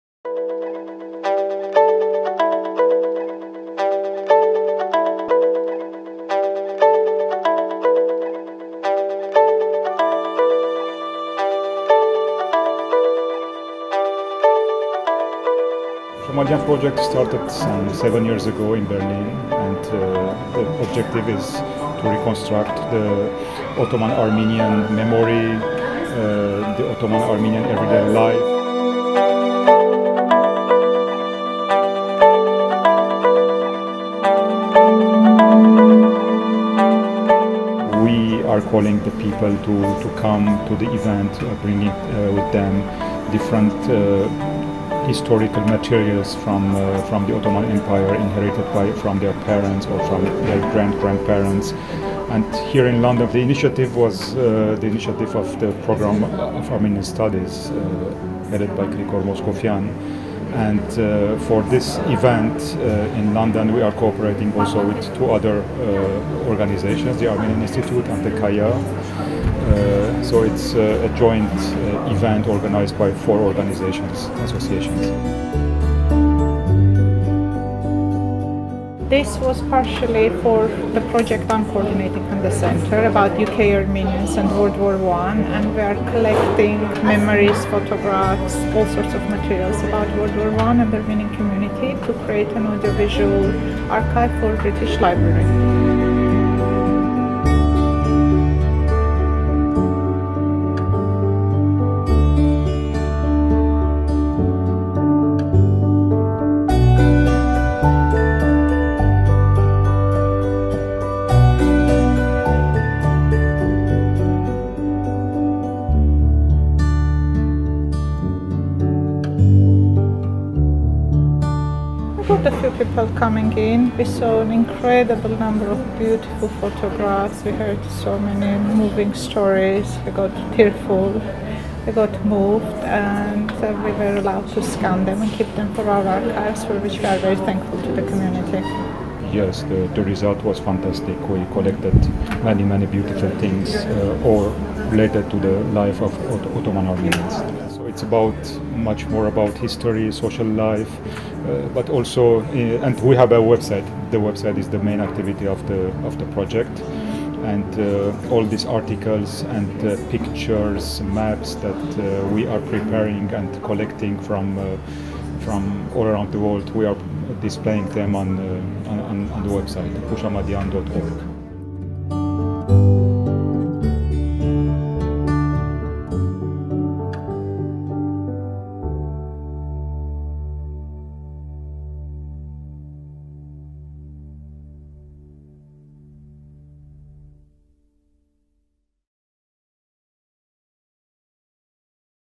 Collection of Objects of Memory
On 4 June, 2017 CAIA hosted an event with Houshamadyan, the Programme of Armenian Studies, and the Armenian Institute to collect old family archives in order to digitise them.

Filmmaker Seda Grigoryan captured some moments from theis moving and interesting event .